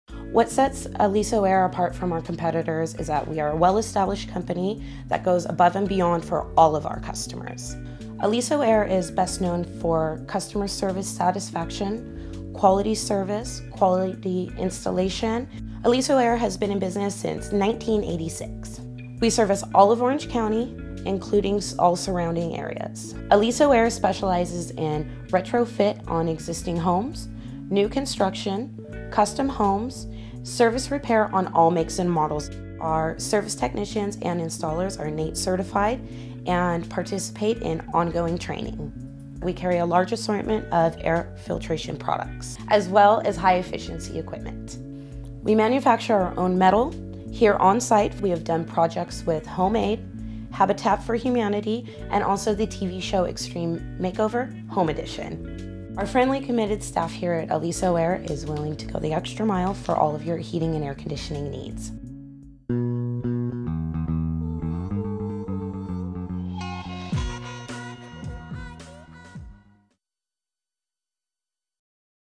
Rancho Santa Margarita Aliso Air Conditioning & Heating
Keep comfy, save money and be energy-efficient with Aliso Air Conditioning & Heating. Serving all of California's Orange County, their NATE-certified techs offer expert retrofitting, installation, repairs and replacement of air quality systems. See why everyone from Habitat For Humanity to Extreme Makeover: Home Edition trusts Aliso to help them breath easier.